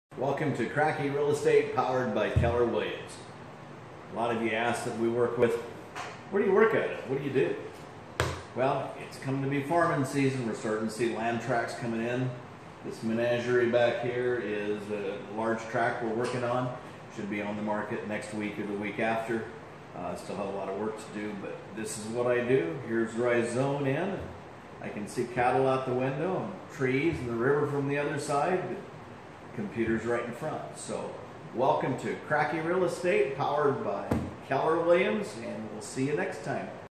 Dons office
Ever wonder what Kracke Real Estate Powered by Keller Williams is all about?  Don will tell you.